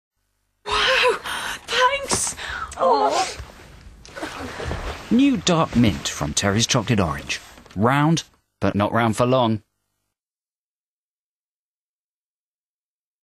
Terry's Chocolate Orange - Minty
Dazzling Dark Mint Commercial.

Actress: Frances Burnett